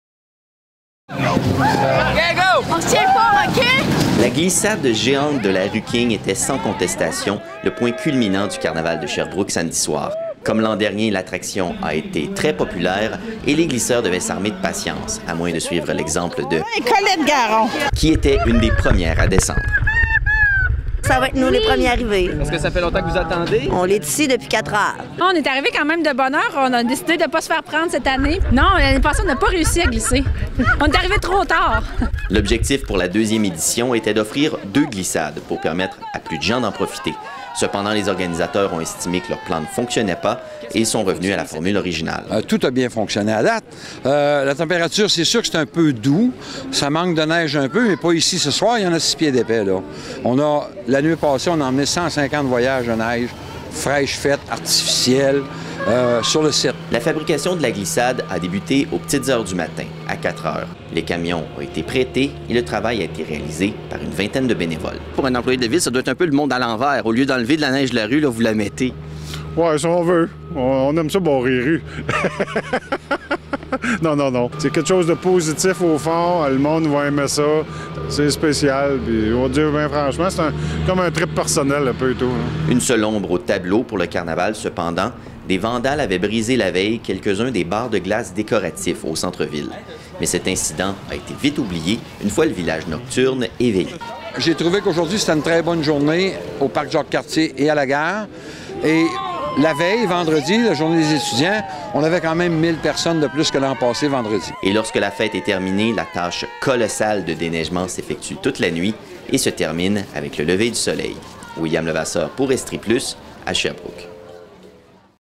Carnaval de Sherbrooke : les 24 heures d'une glissade au centre-ville - Estrie Plus
À 4 h du matin samedi, une vingtaine de bénévoles et d'employés municipaux envahissaient la côté de la rue King de Sherbrooke avec comme mission de recouvrir une des artères principales de la ville avec de la neige.